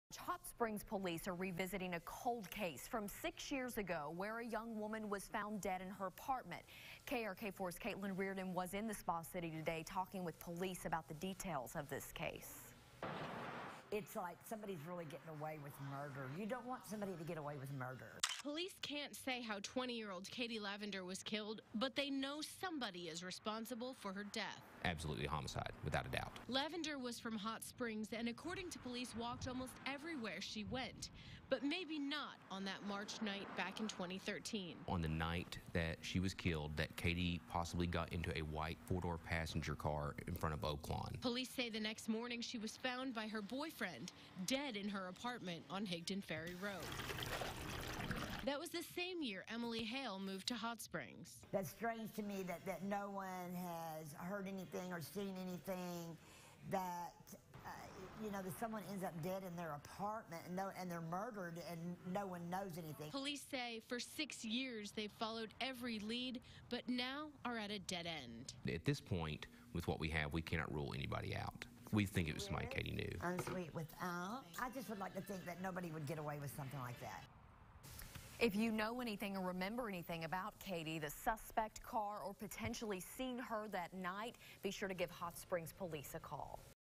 Hot Springs Police Looking for New Leads in 2013 cold case
Hot springs PD looking for new leads in 2013 cold case where a 20 year old woman was found dead in her apartment